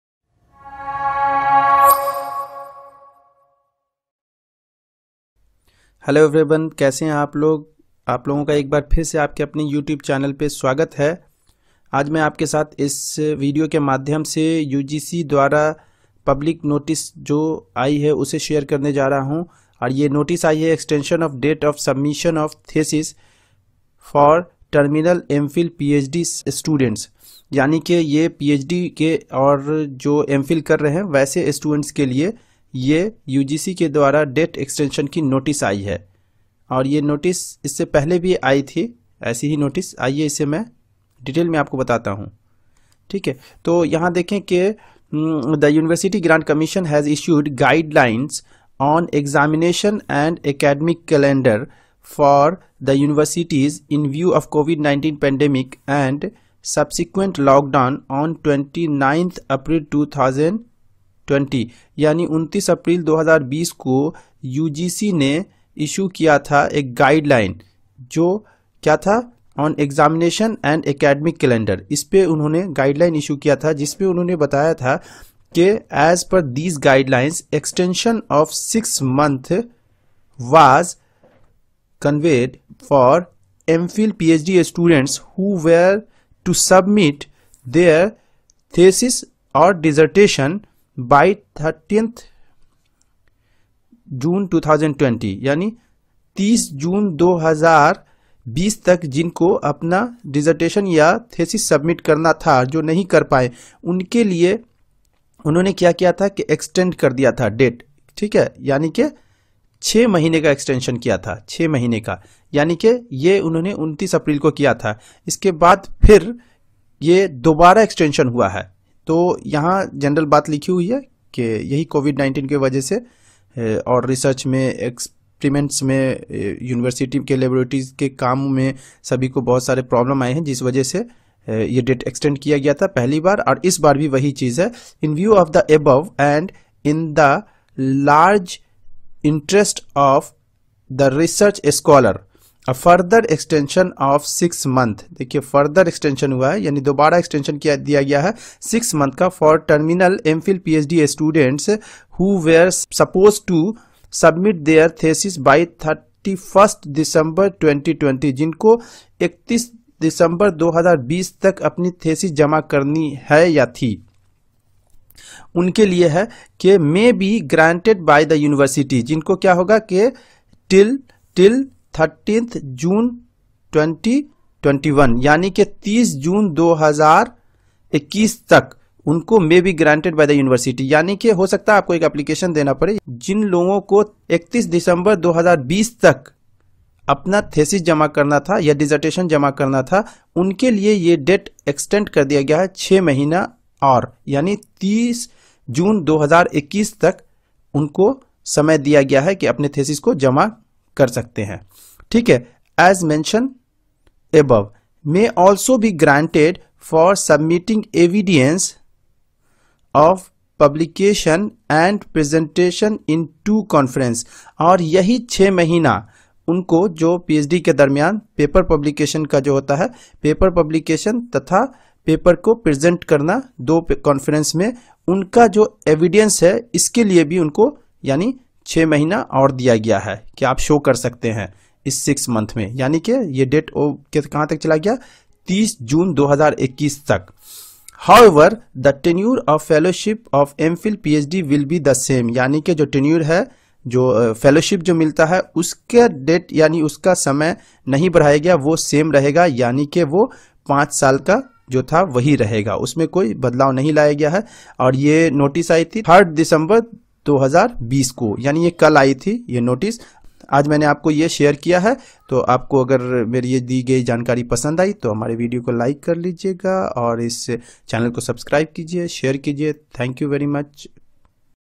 Ph.D/M.Phil thesis submission date extension I UGC Public Notice 2020
Extension of date for submission of thesis for terminal M.Phil/Ph.D students